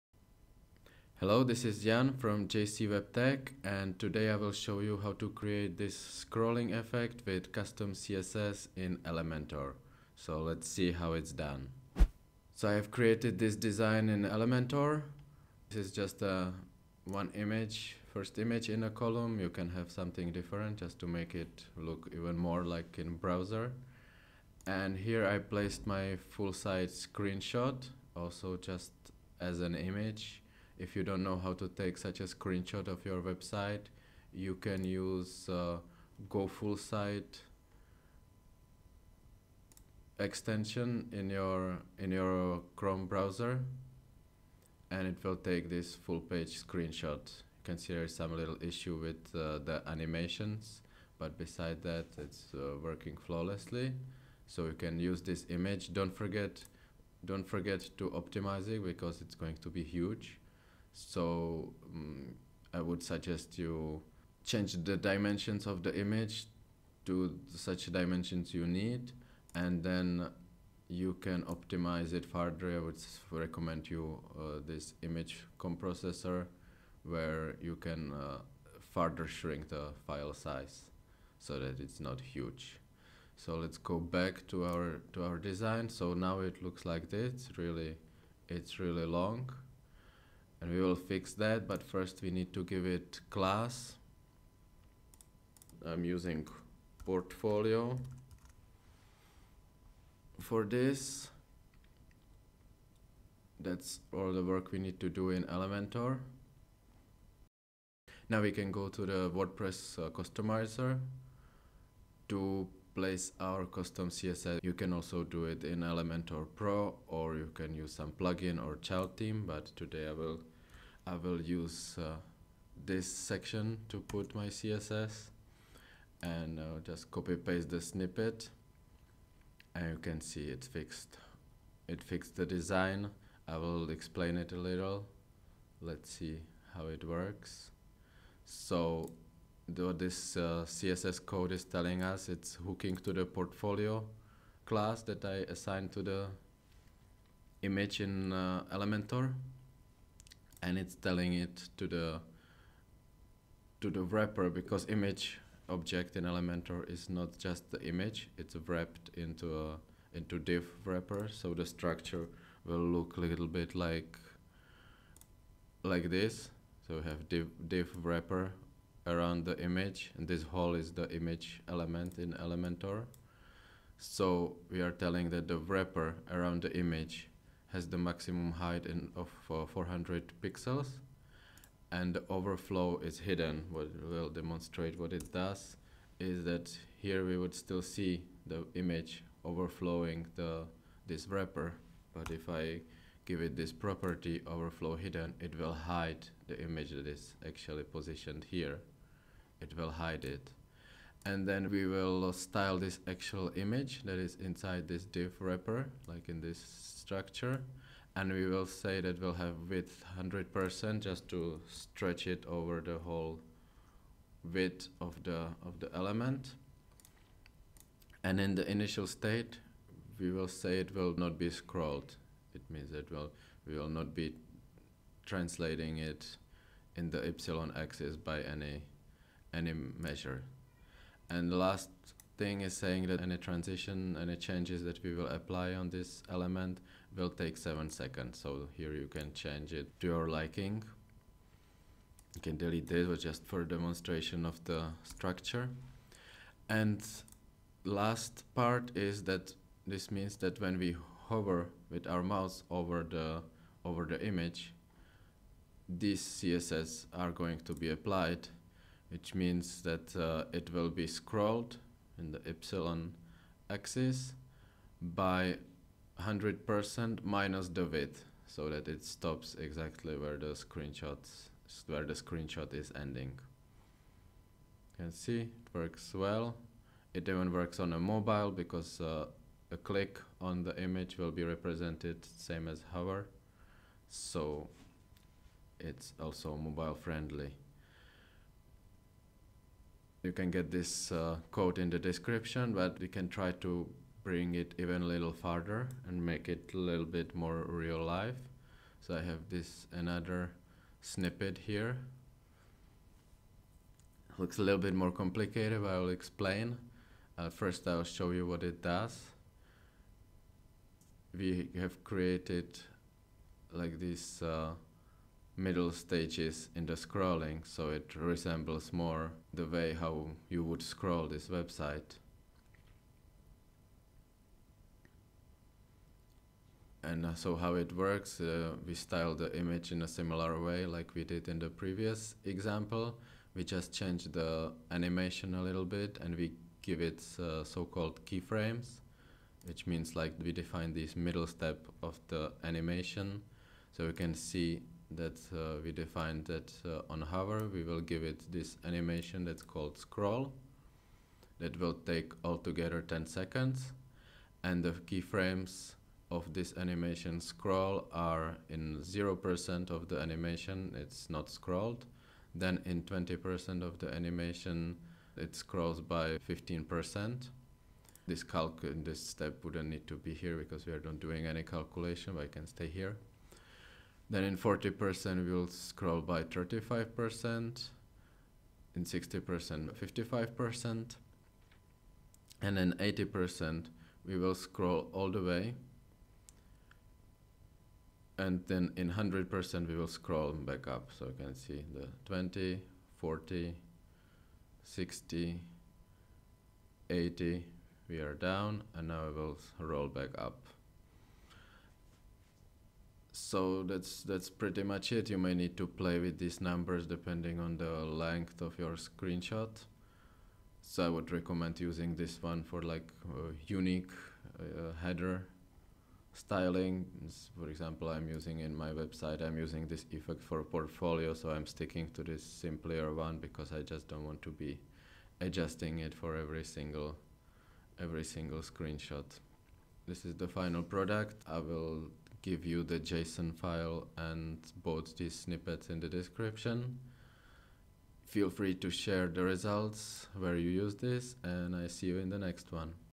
CSS hover scrolling effect in ELEMENTOR
Have you ever wondered how to create this CSS-only scroll hover animation where an image of a website scrolls up and down when you hover over it with your mouse?

You will find the code and JSON ELEMENTOR file in my blog post:

I am using it for example in my portfolio.